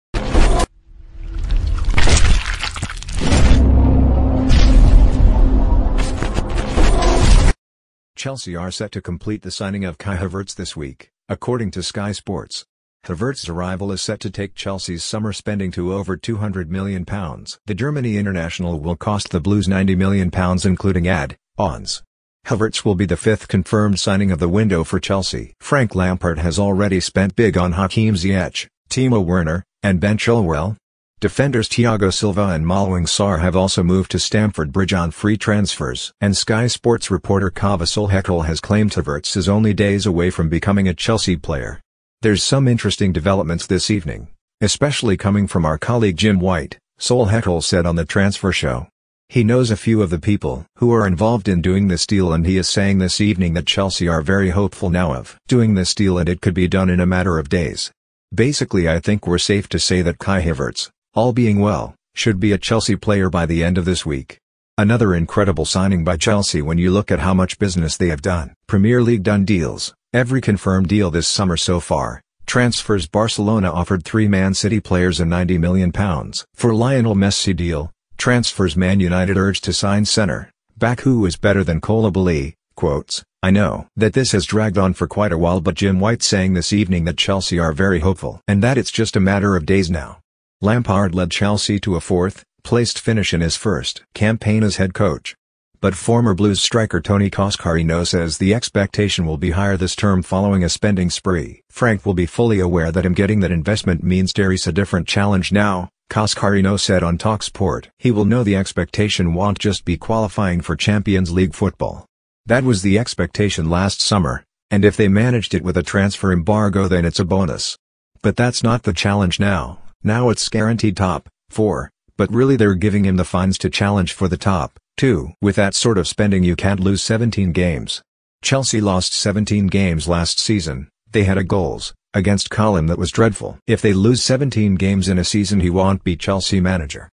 Chelsea to make Kai Havertz transfer announcement 'this week' with deal almost done
Chelsea are set to complete the signing of Kai Havertz this week, according to Sky Sports. Havertz's arrival is set to take Chelsea's summer spending to over £200million. The Germany international will cost the Blues £90m including add-ons. Havertz will be the fifth confirmed signing of the window for Chelsea. Frank Lampard has already spent big on Hakim Ziyech, Timo Werner and Ben Chilwell. Defenders Thiago Silva and Malang Sarr have also moved to Stamford Bridge on free transfers. And Sky Sports reporter Kaveh Solhekol has claimed Havertz is only days away from becoming a Chelsea player. "There's some interesting developments this evening, especially coming from our colleague Jim White," Solhekol said on The Transfer Show. "He knows a few of the people who are involved in doing this deal and he is saying this evening that Chelsea are very hopeful now of doing this deal and it could be done in a matter of days. "Basically I think we're safe to say that Kai Havertz, all being well, sho